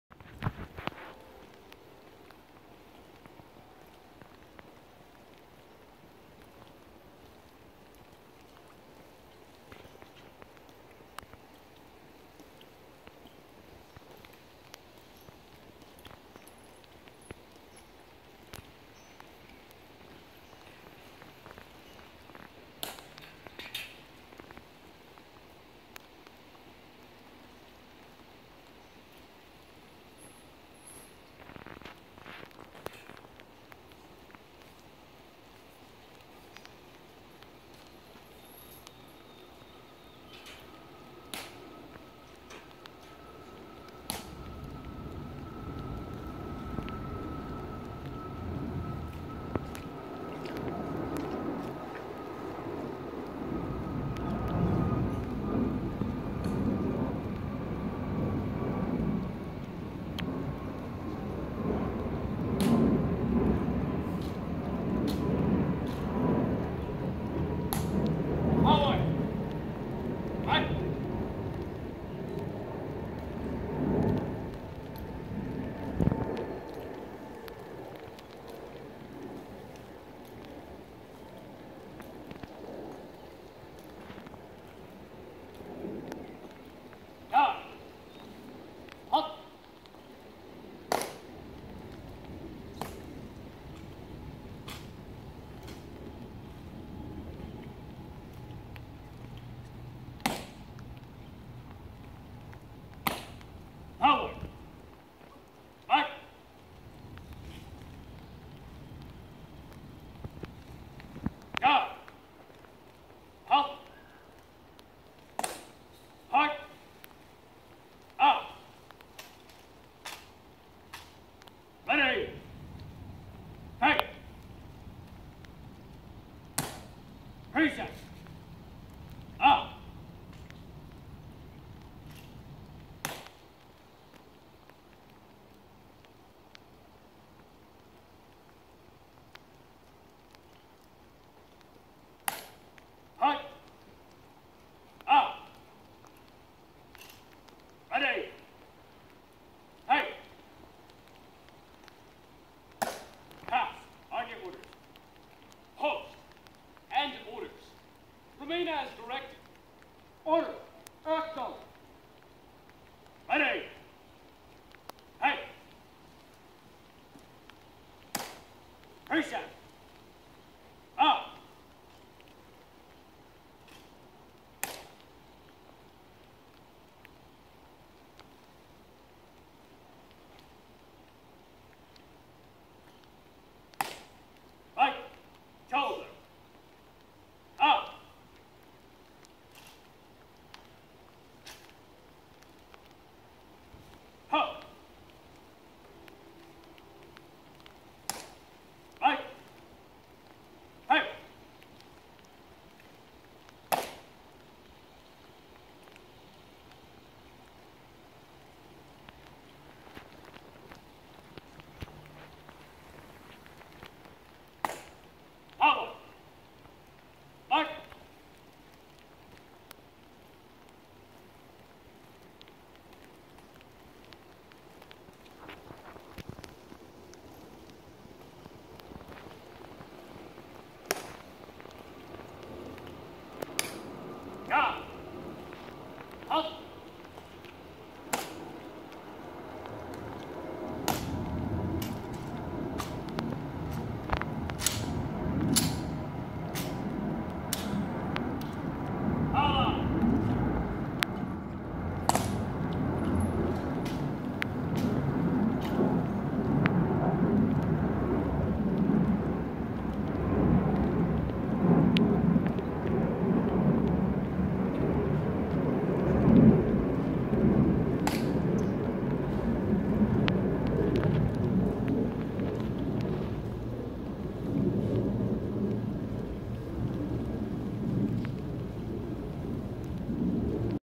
Changing Sentinel Guards at Tomb of the Unknowns (2019)
3rd U.S. Infantry Regiment - Changing Sentinel Guards watching over the Tomb of Unknown Soldier